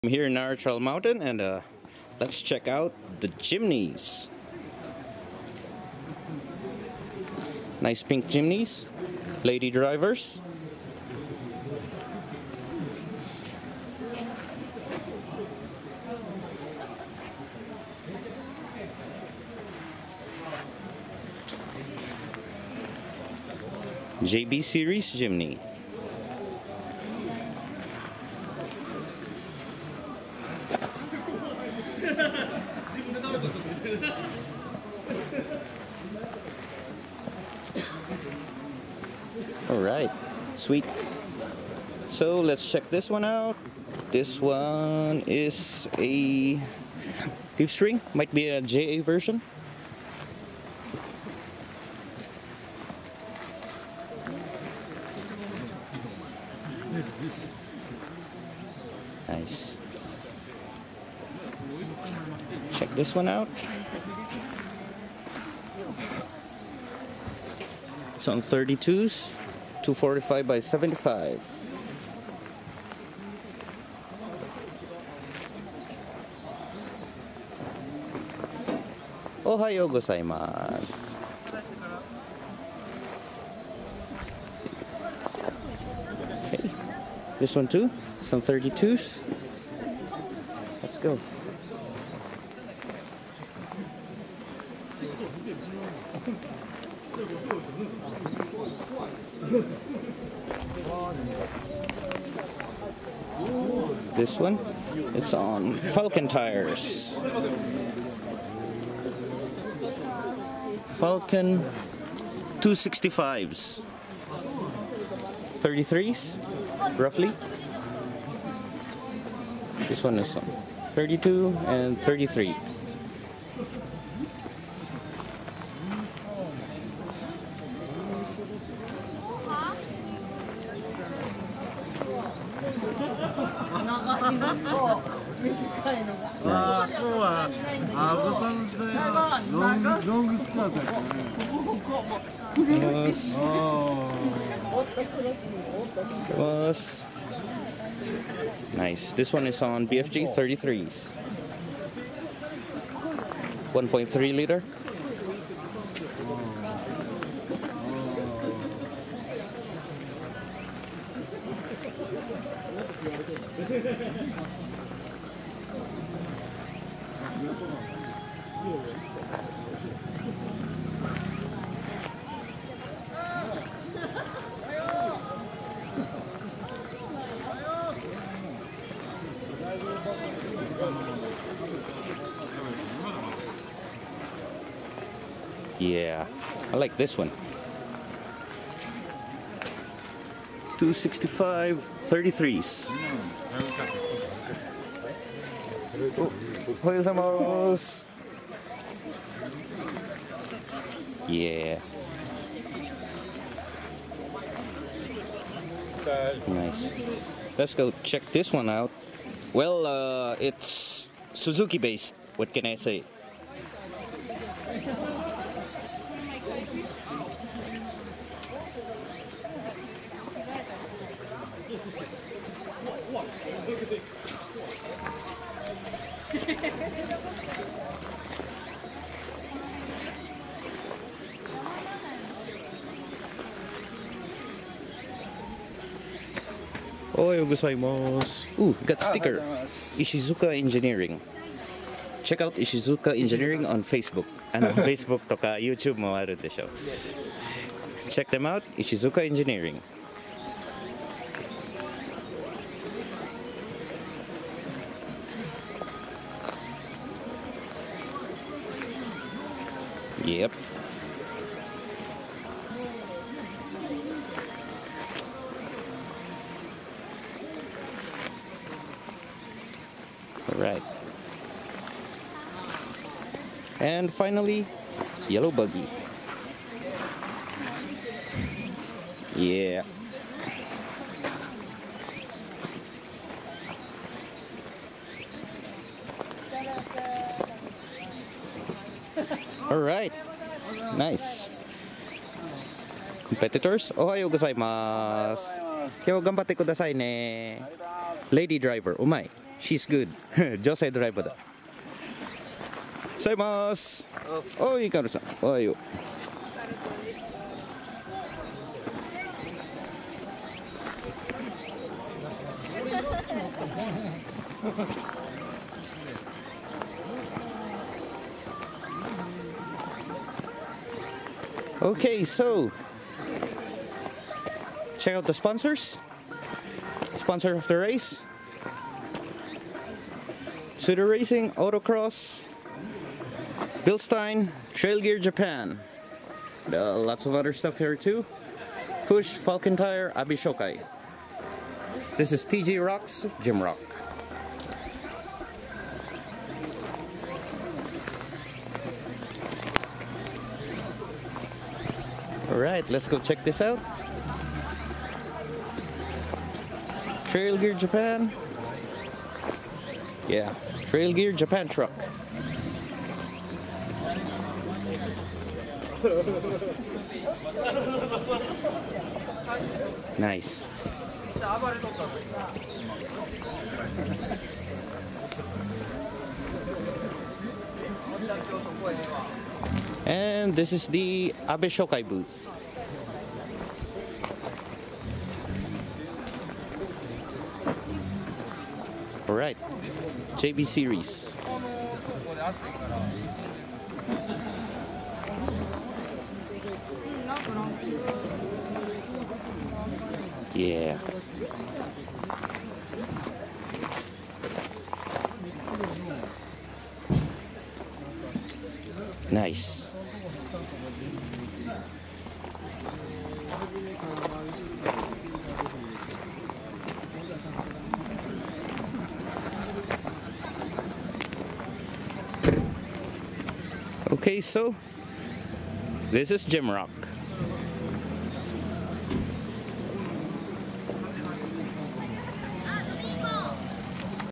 ジムニー ⚫ ジムロック Jimrock Suzuki Jimny
This event is an all Suzuki Jimny 4x4 rock crawling race held in Nara trial mountain, Uda city Nara prefecture Japan.